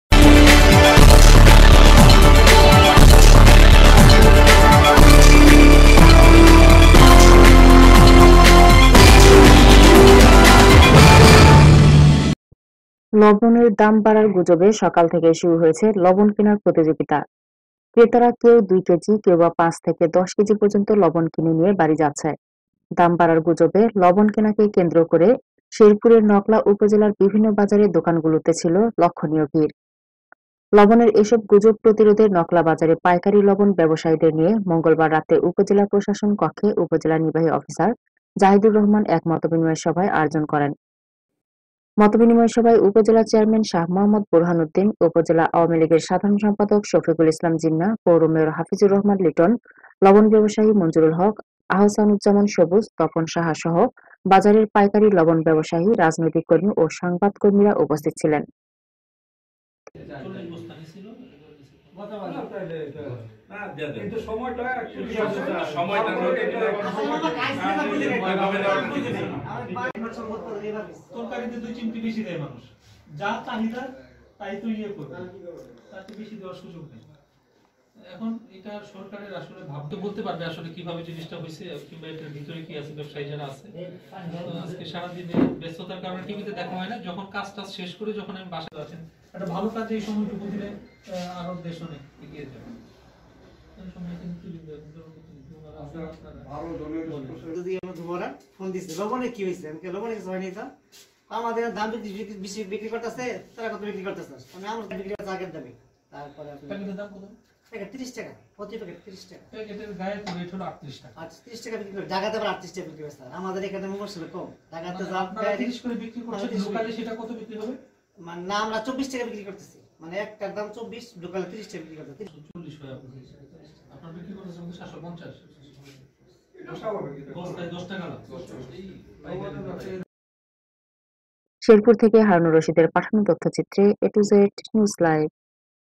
নকলায় লবণের গুজব রোধে ব্যবসায়ীদের সাথে মতবিনিময়|Sherpur|a2znewslivetv
লবণের দাম বাড়ার গুজবে সকাল থেকেই শুরু হয়েছে লবণ কেনার প্রতিযোগিতা। ক্রেতারা কেউ দুই কেজি কেউবা পাঁচ থেকে দশ কেজি পর্যন্ত লবণ কিনে নিয়ে বাড়ি যাচ্ছে। দাম বাড়ার গুজবে লবণ কেনাকে কেন্দ্র করে শেরপুরের নকলা উপজেলার বিভিন্ন বাজারের দোকানগুলোতে ছিল লক্ষণীয় ভীড়।
লবণের এসব গুজব প্রতিরোধে নকলা বাজারের পাইকারী লবণ ব্যবসায়ীদের নিয়ে মঙ্গলবার রাতে উপজেলা প্রশাসন কক্ষে উপজেলা নির্বাহী অফিসার জাহিদুর রহমান এক মতবিনিময় সভার আয়োজন করে। মতবিনিময় সভায় উপজেলা চেয়ারম্যান শাহ মোঃ বোরহান উদ্দিন, উপজেলা আওয়ামী লীগের সাধারণ সম্পাদক শফিকুল ইসলাম জিন্নাহ, পৌর মেয়র হাফিজুর রহমান লিটন, লবণ ব্যবসায়ী মুঞ্জুরুল হক, আহসানুজ্জামান সবুজ, তপন সাহাসহ বাজারের পাইকারী লবণ ব্যবসায়ী, রাজনৈতিক কর্মী ও সংবাদকর্মীরা উপস্থিত ছিলেন।